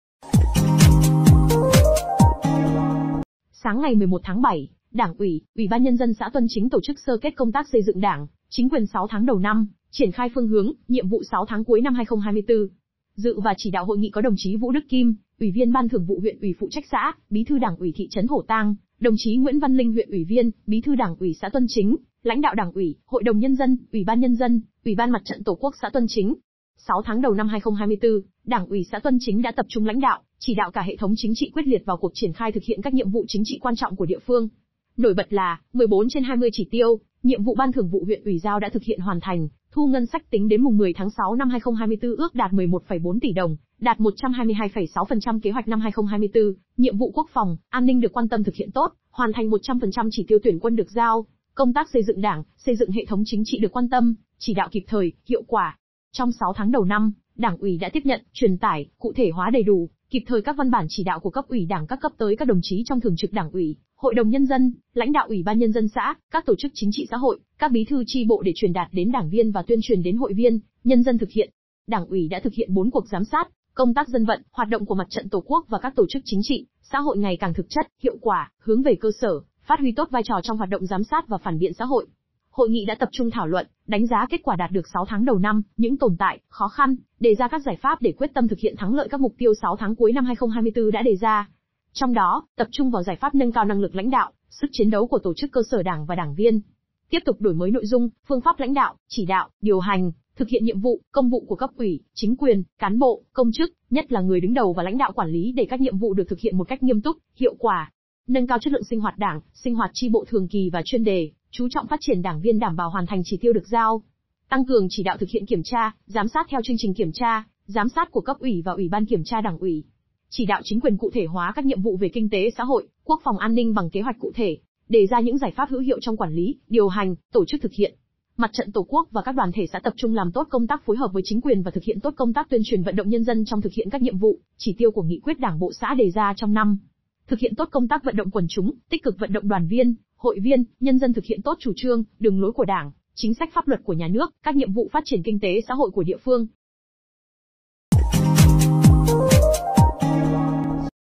VIDEO: Xã Tuân Chính sơ kết công tác xây dựng Đảng, chính quyền 6 tháng đầu năm.
Sáng ngày 11/7, Đảng ủy, UBND xã Tuân Chính tổ chức sơ kết công tác xây dựng Đảng, chính quyền 6 tháng đầu năm; triển khai phương hướng, nhiệm vụ 6 tháng cuối năm 2024. Dự và chỉ đạo hội nghị có đồng chí Vũ Đức Kim – Ủy viên BTV Huyện ủy phụ trách xã, Bí thư Đảng ủy Thị trấn Thổ Tang; đồng chí Nguyễn Văn Linh-Huyện ủy viên, Bí thư Đảng ủy xã Tuân Chính; lãnh đạo Đảng ủy, HĐND, UBND, Uỷ ban MTTQ xã Tuân Chính.